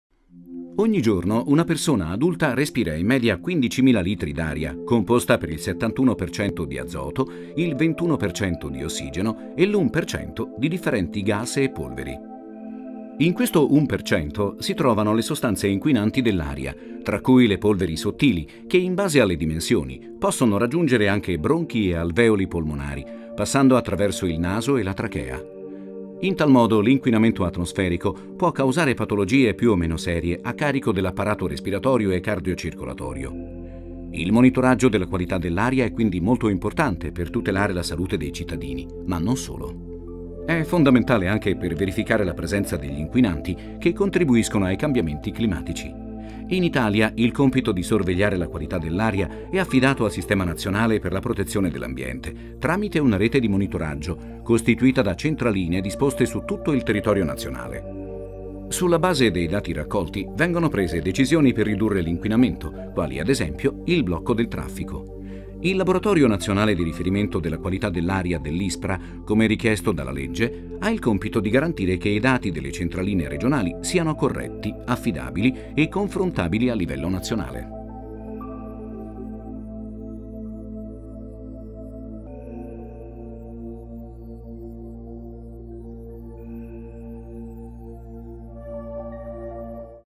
Metrologia ambientale - La qualità dell'aria
Un minuto del tuo tempo per iniziare a scoprire cosa fa la ricerca scientifica per tutti noi.
Esplora i laboratori dell'ISPRA nel tour virtuale realizzato per la Notte Europea dei Ricercatori